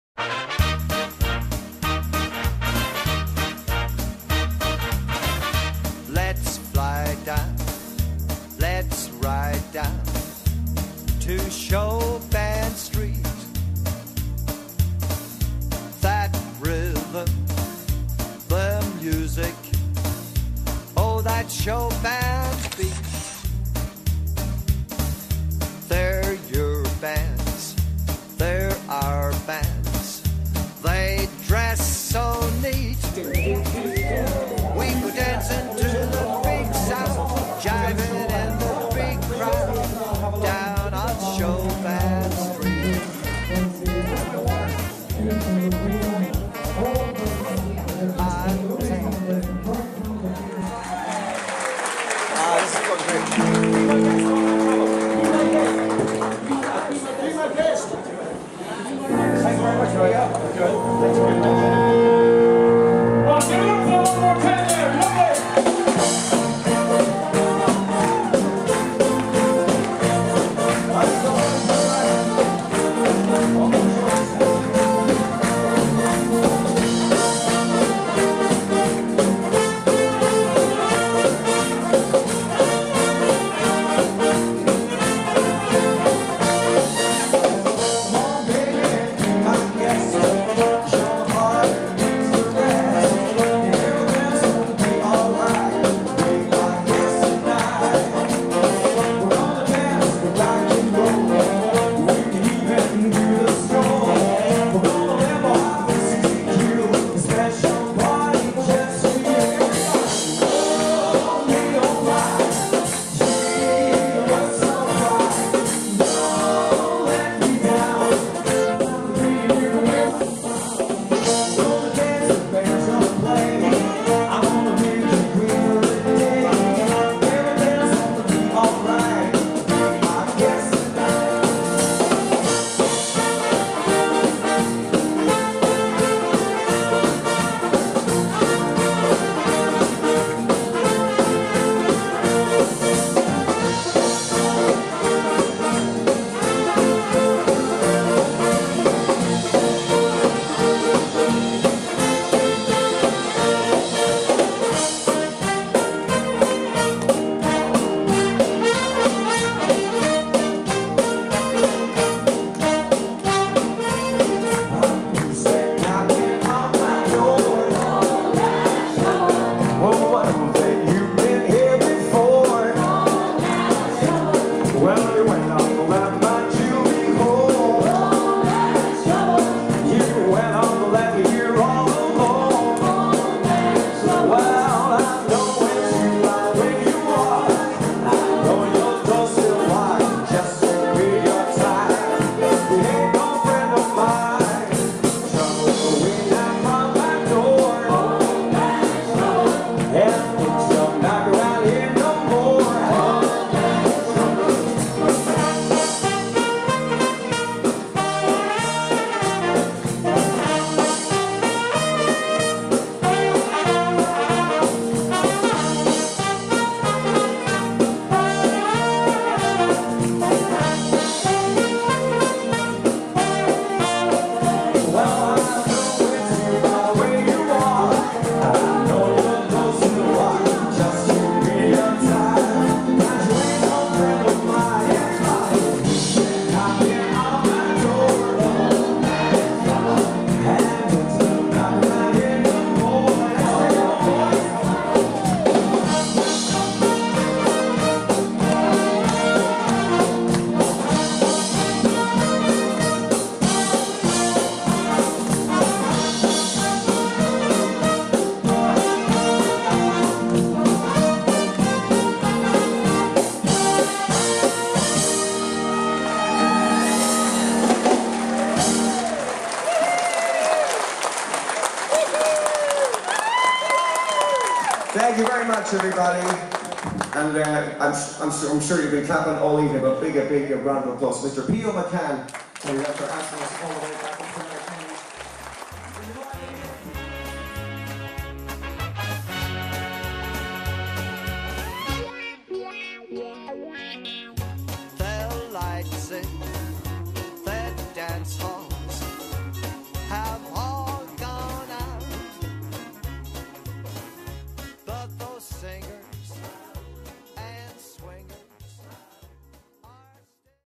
Highland Radio Concert 1
Keith & Lorraine McDonald, The Showband Show, performing at the Highland radio Christmas Concert 2015